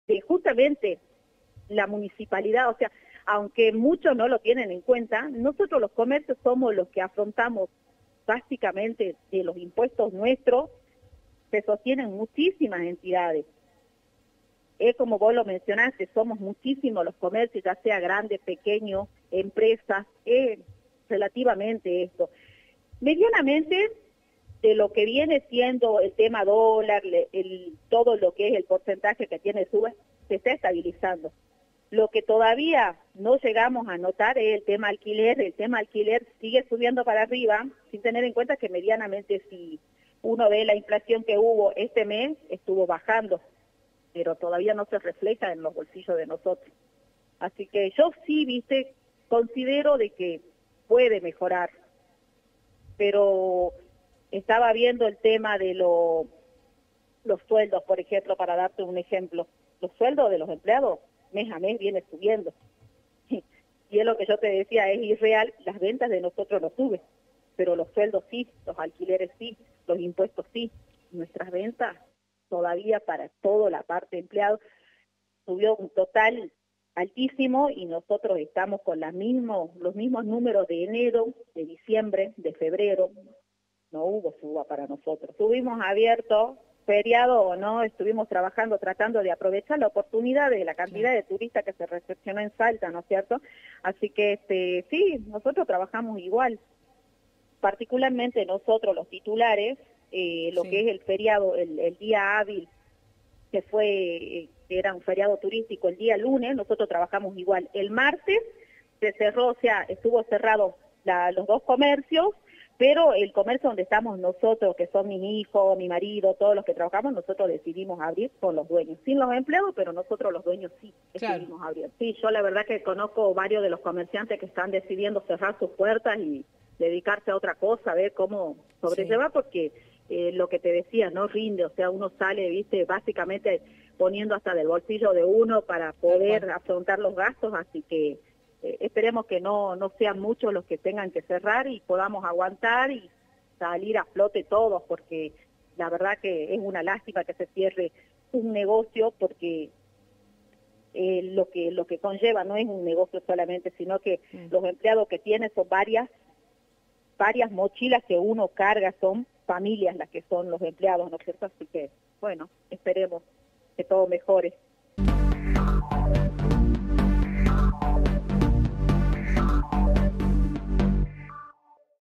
SALTA - Profunda crisis en el comercio, subas de los alquileres y bajas de las ventas
Com. Tel. Silvia - comerciante